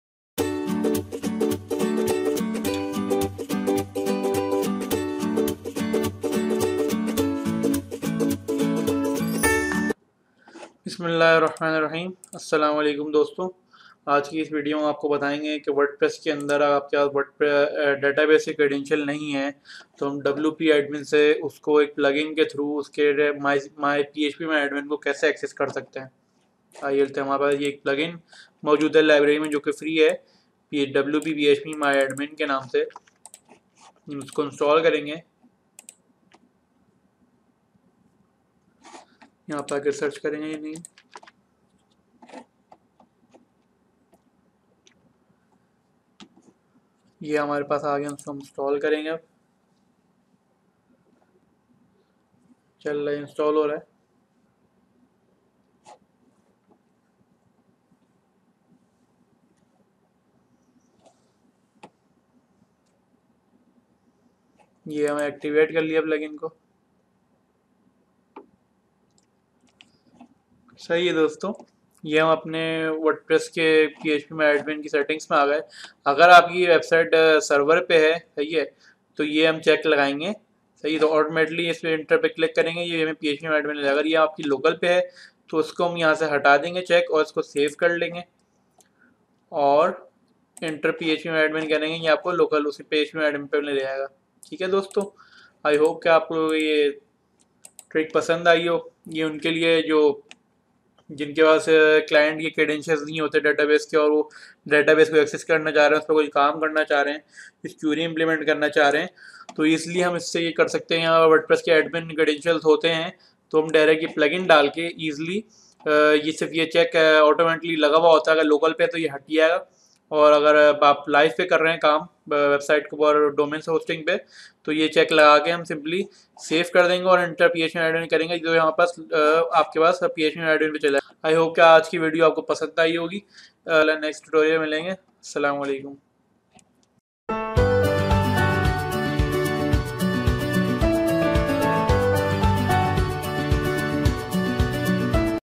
How To Access Phpmyadmin From WordPress admin | wordpress tutorial for beginners | In 2021
In this video you can learn how to access  Phpmyadmin from Wordpress Admin very easy.